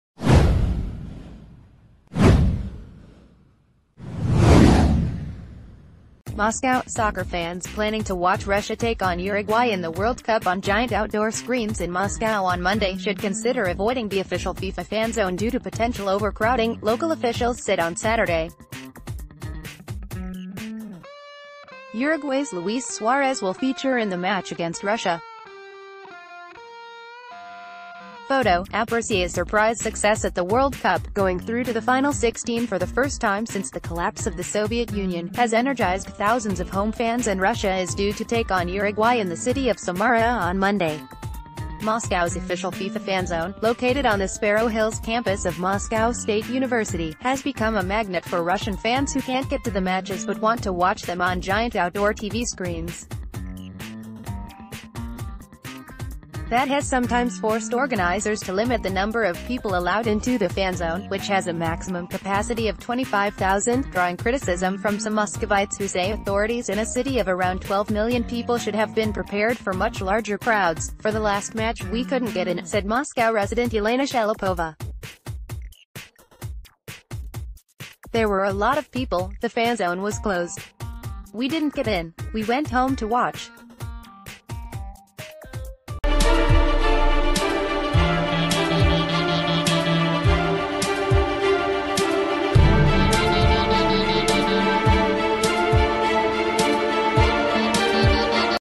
Fanzone overcrowding fear as World Cup fever sweeps Russia
Moscow: Soccer fans planning to watch Russia take on Uruguay in the World Cup on giant outdoor screens in Moscow on Monday should consider avoiding the official FIFA fan zone due to potential overcrowding, local officials said on Saturday. Uruguay's Luis Suarez will feature in the match against Russia. Photo: APRussia's surprise success at the World Cup, going through to the final-16 for the first time since the collapse of the Soviet Union, has energised thousands of home fans and Russia is due...